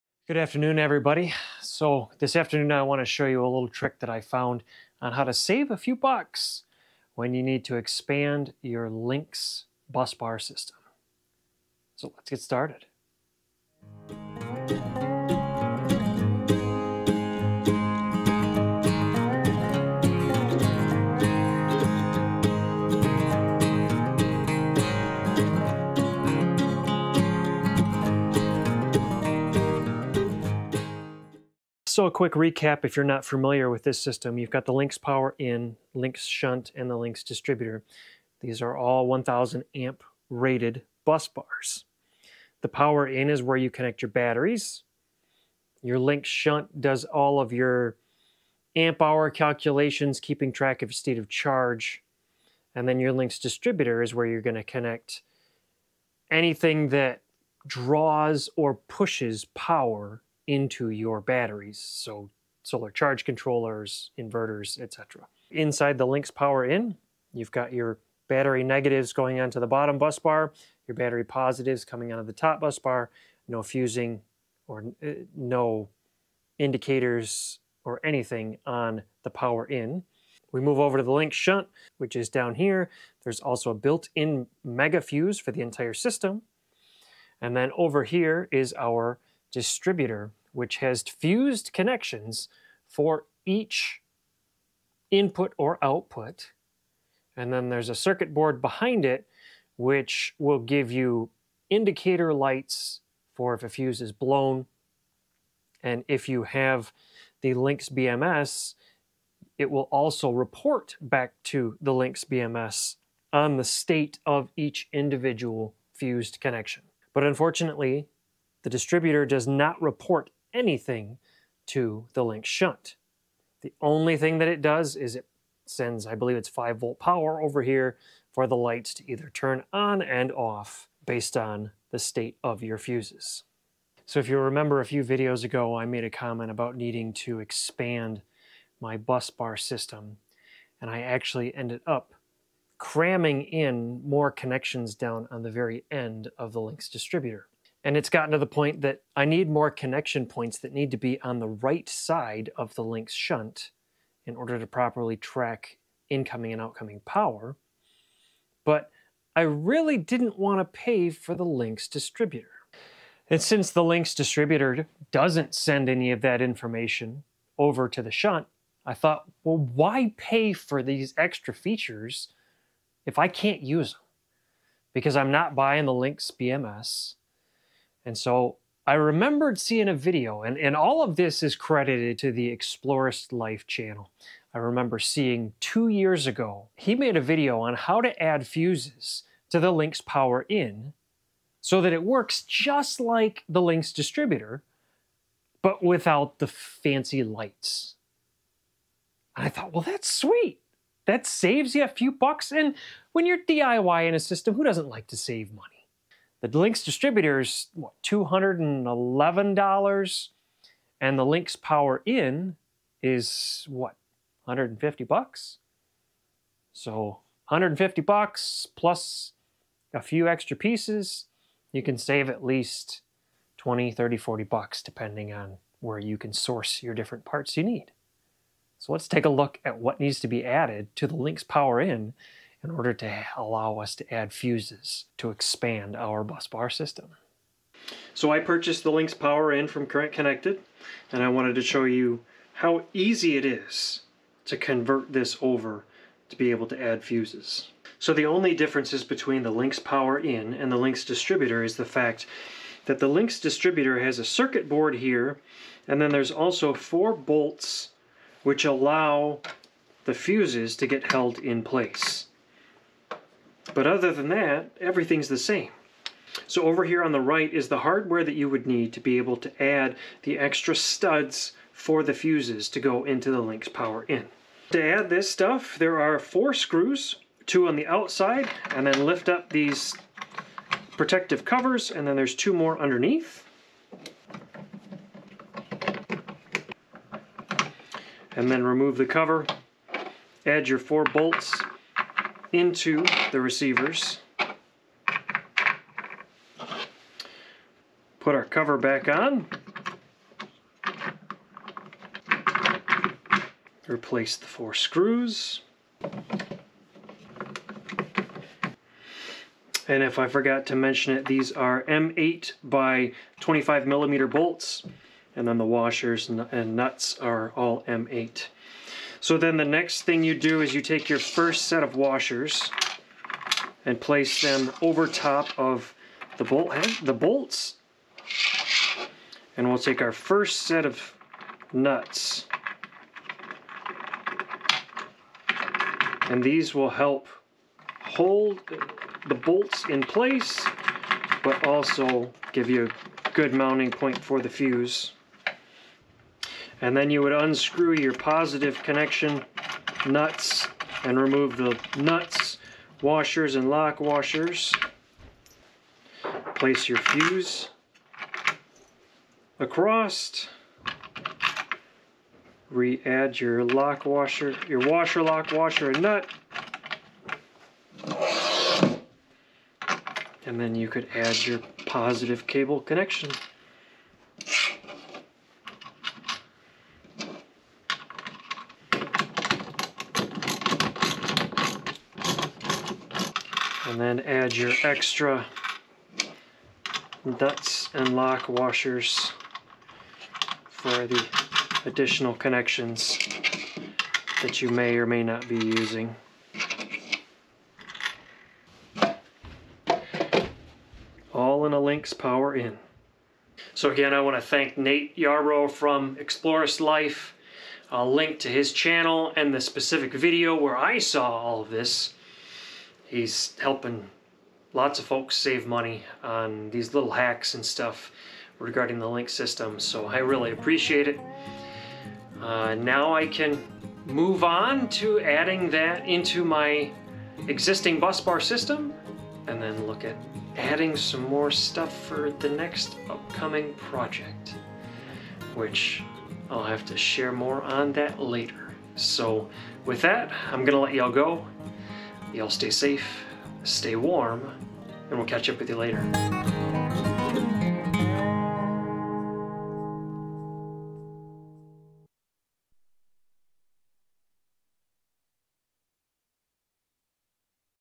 Saving Some Money by Fusing a Victron Lynx Power-In
Looking to expand your Victron Lynx bus bar system without breaking the bank? In this video, we'll show you a cost-effective solution by adding fuses to your setup saving you from needing to buy a Lynx Distributor.
By incorporating fuses, you can safeguard your equipment from potential overloads and shorts, ultimately saving you money on repairs and replacements.
Join us as we guide you through the simple process of installing fuses to enhance the safety and longevity of your Victron Lynx Power-In system.

M8 x 25mm Hex Head Screw Bolt

M8 Stainless Steel Flat Washer

M8 Copper Split Lock Washer

M8 Hex Nut